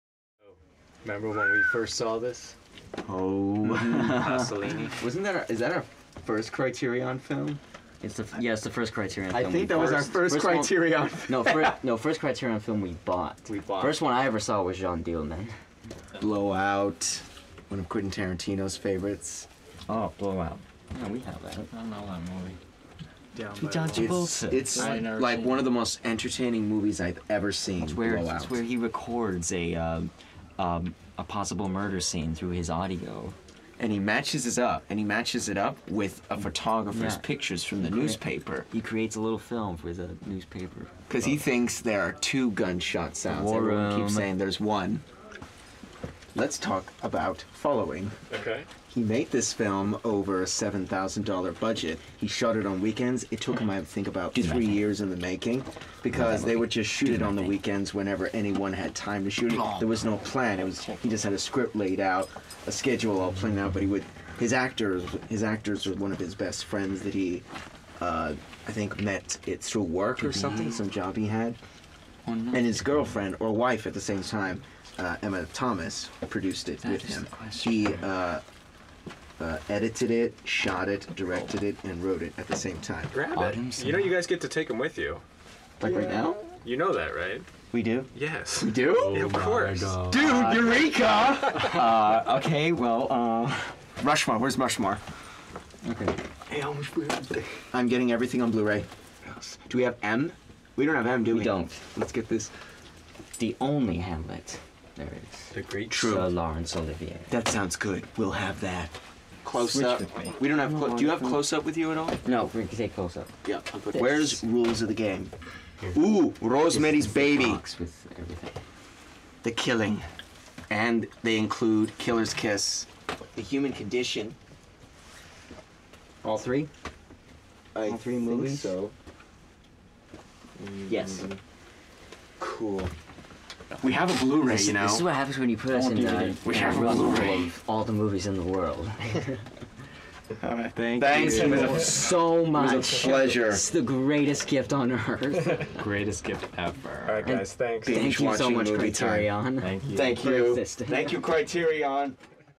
The Wolfpack - DVD Picks
The Wolfpack stopped by the Criterion offices for a screening and had the time of their lives in the DVD closet!